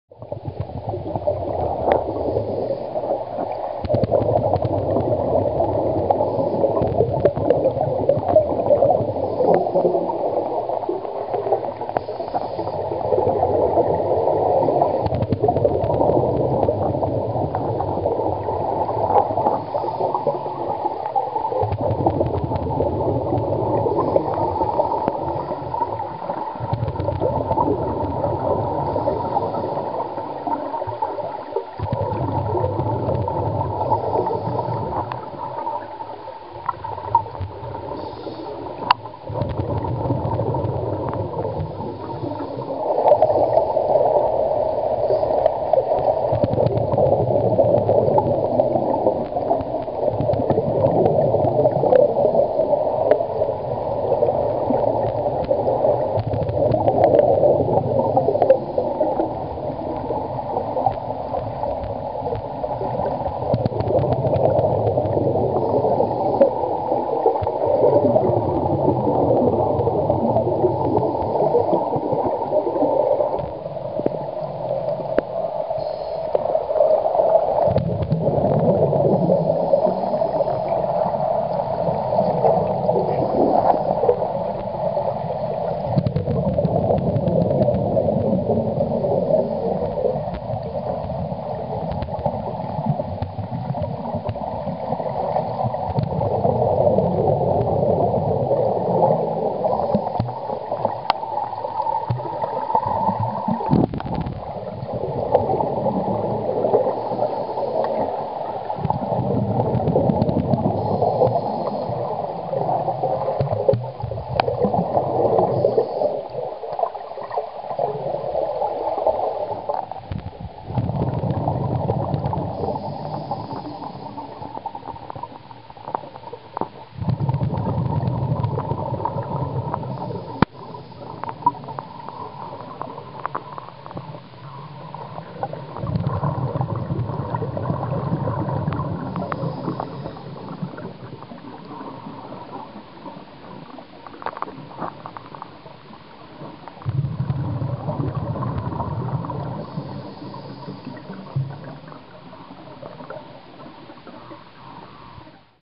Uitzet Beekforel 2015
On March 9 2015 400 Kilograms of brown trout were stocked in Lake Oostvoorne (Netherlands) by sportfishing federation 'Sportvisserij Zuidwest Nederland'